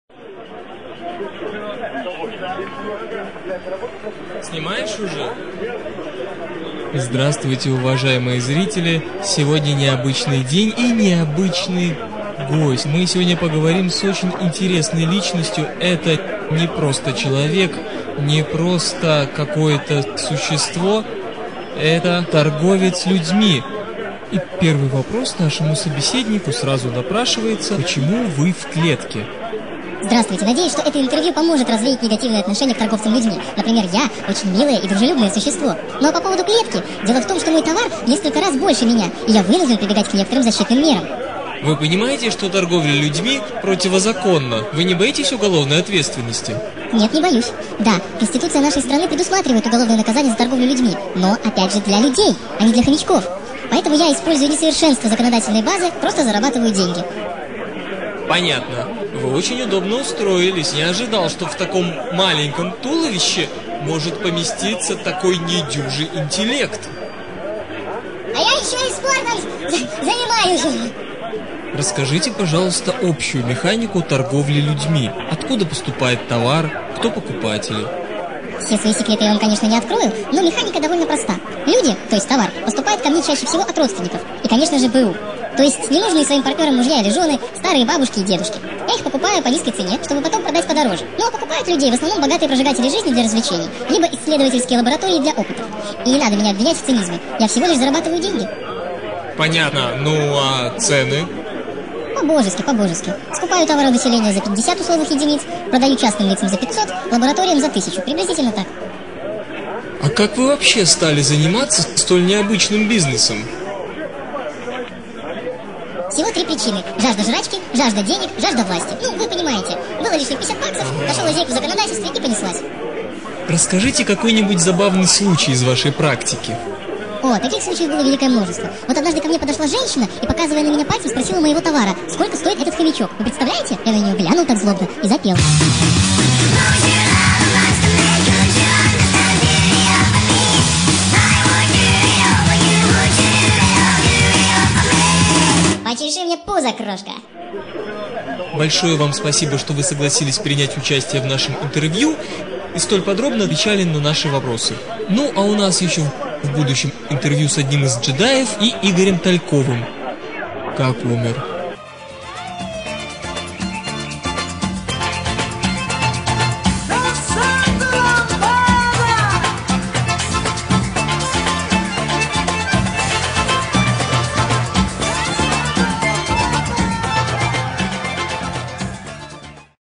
Интервью с торговцем людьми
Мы долго его искали. Долго упрашивали. И вот, торговец людьми прямо перед камерой честно ответить на все наши вопросы.

Высказать свое мнение о ролике и посмотреть другие можно в моем блоге